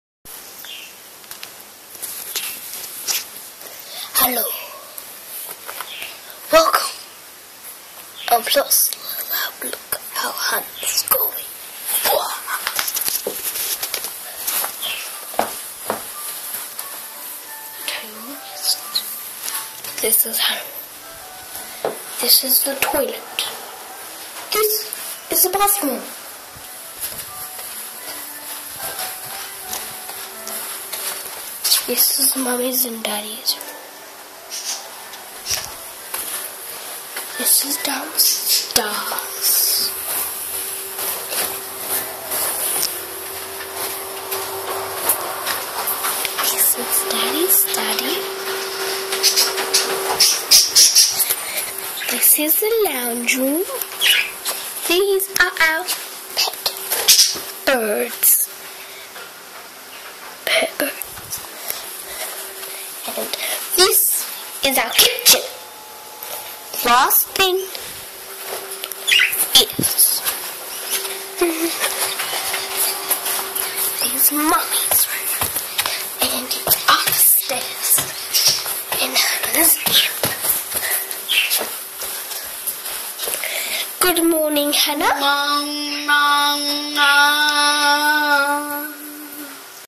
A video adventure through our messy house
Eli wanders around the house with my smartphone. Gripping stuff.